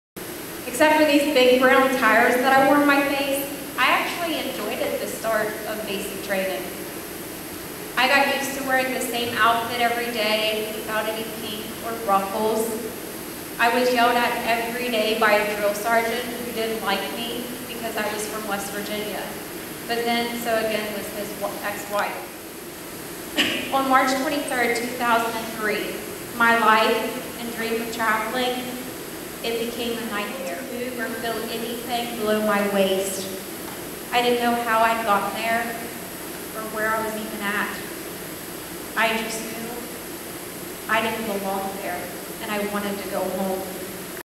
Jessica Lynch speaks to school
Former POW Jessica Lynch speaks to students in Williamstown, WV.